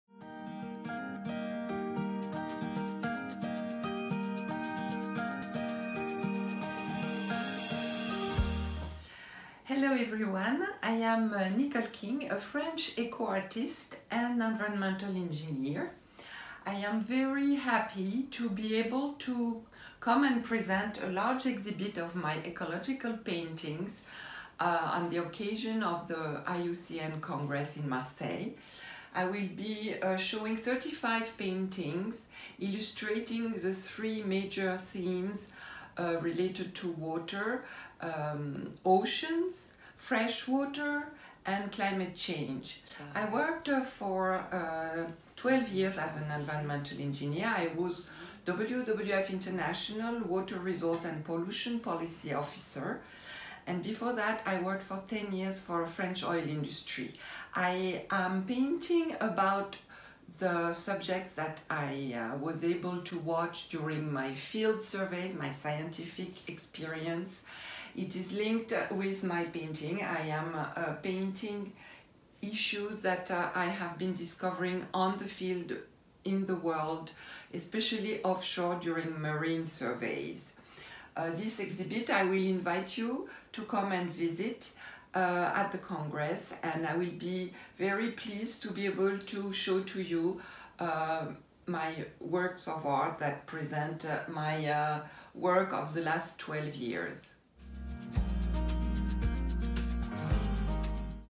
Nicole King is part of the official IUCN Congress program !
Nicole King is invited to be part of the official program : she proposes a cultural event, an exhibition of 35 paintings "Argument for water's survival" at the occasion of the World Conservation Congress organized by IUCN (International Union for Conservation of Nature) from September 3rd to 11th, 2021. This exceptional event for ecology is hosted  by France for the first time in 70 years.